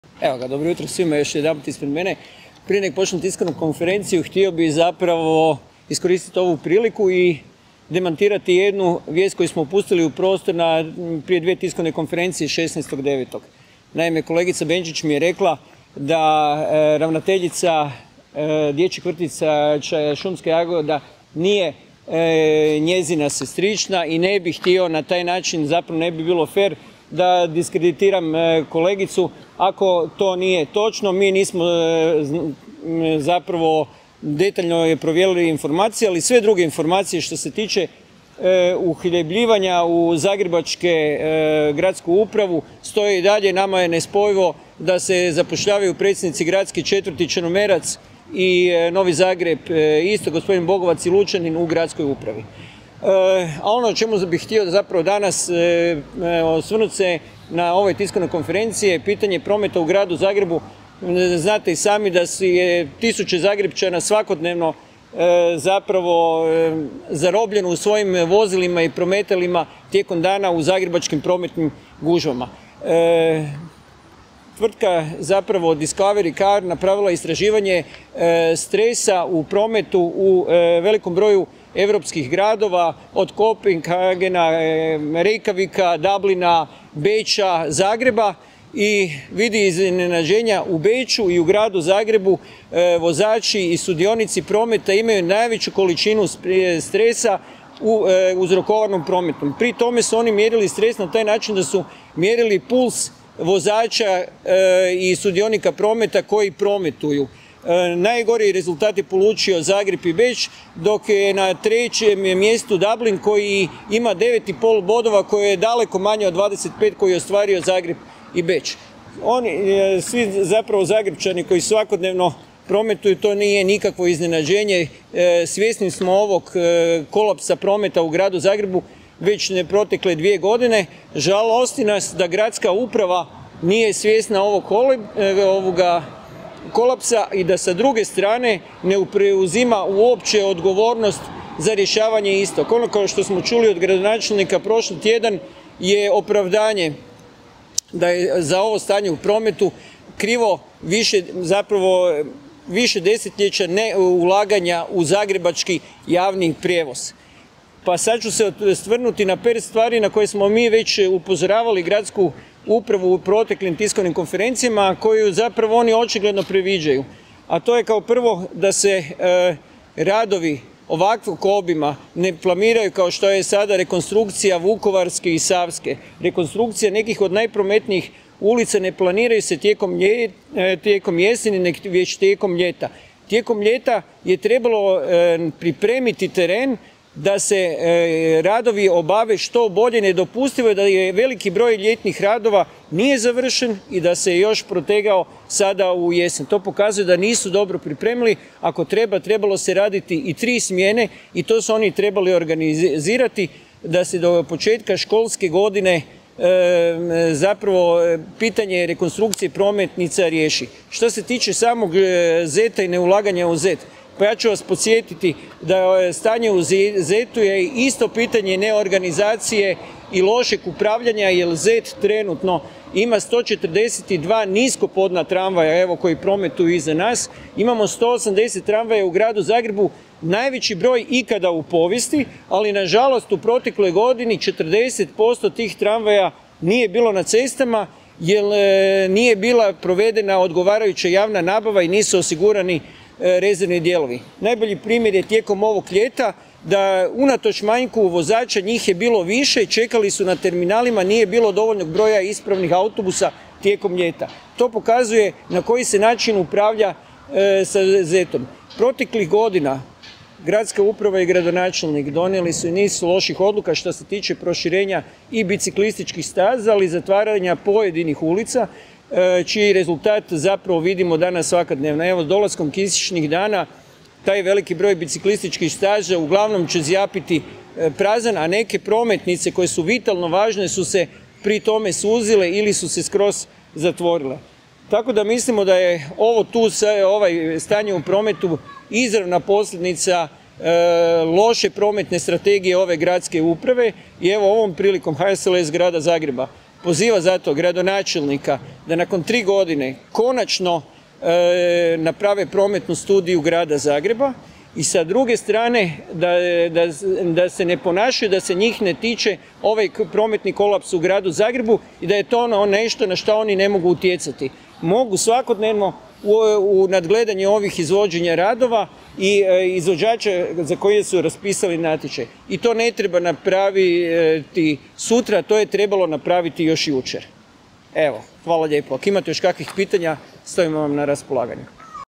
Darko Klasić o prometnom kolapsu u Gradu Zagrebu
Na konferenciji za medije stranke HSLS održanoj na Zrinjevcu o aktualnim temama govorili su saborski zastupnik Darko Klasić i Kristijan Jelić, potpredsjednika HSLS-a Grada Zagreba.